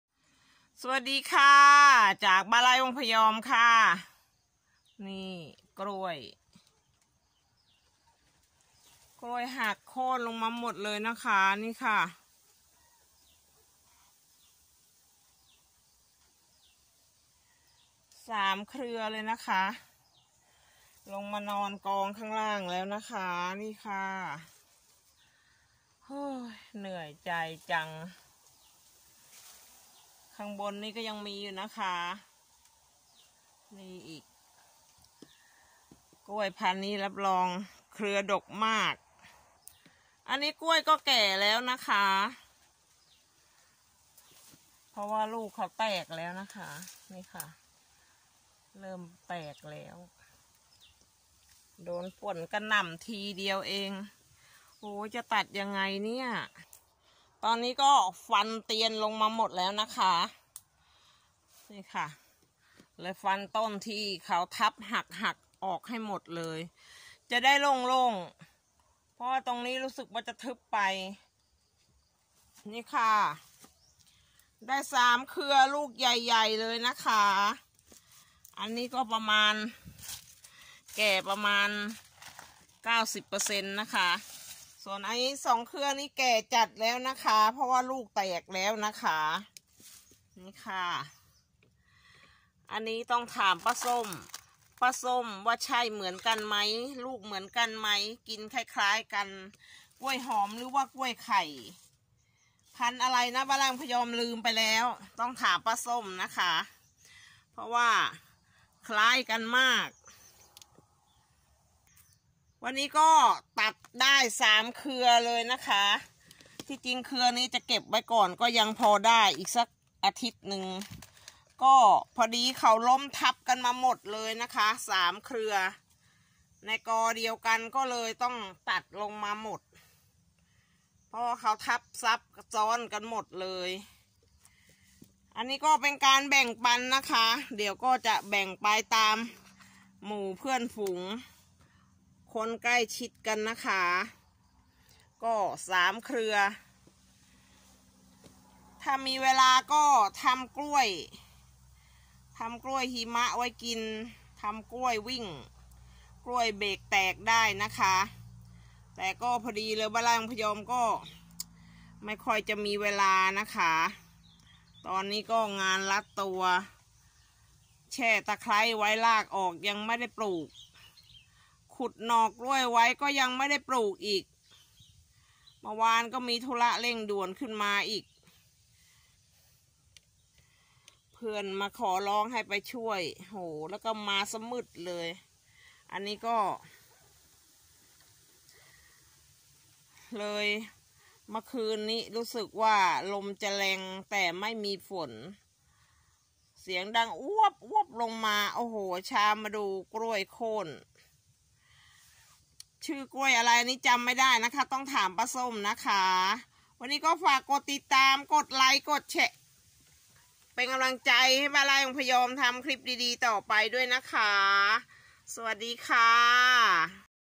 มาแล้วจ้า "กล้วยหอมจอมพล" จากfcเมื่อปีที่แล้วได้สามเครือ!! ที่​💞บ้านไร่​วัง​พ​ยอม​💞
คลิป​นี้​มีคำตอบ​ สำหรับ​มือใหม่​หัด​ปลูก​ต้นไม้​  ปลูก​ในสิ่ง​ที่​ชอบและกินได้  นี่คือความ​สุข​ของ​เรา​